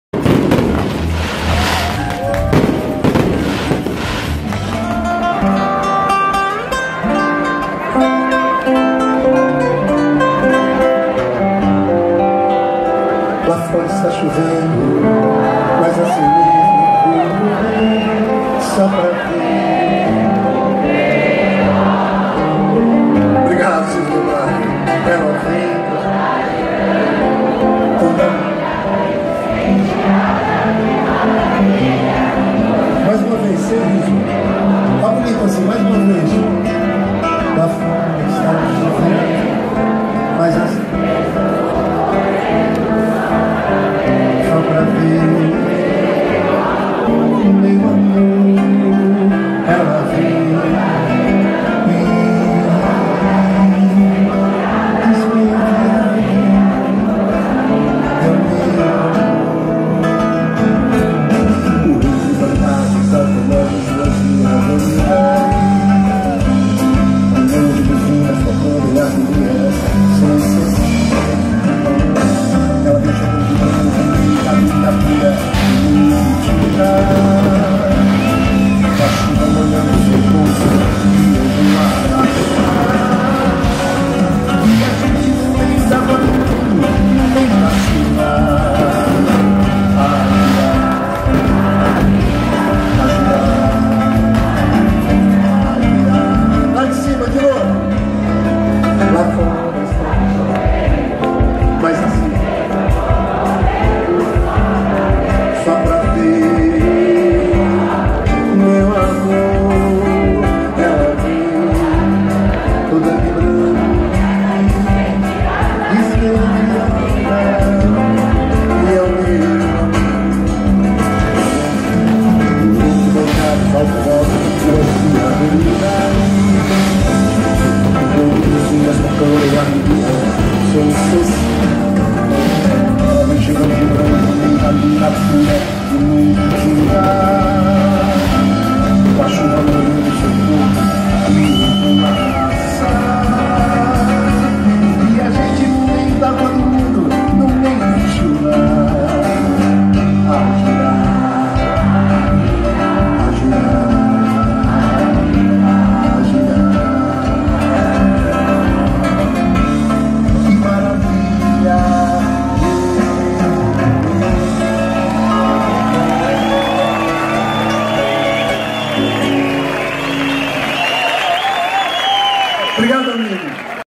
Toquinho no Guarujá
Parabéns Guarujá!
Dr. Valter Suman devolve mais um pouco do brilho à Pérola. Guarujaenses com orgulho de sua cidade. Era esta a sensação que estava no ar nesta festa às vésperas do dia 30, dia do aniversário propriamente dito.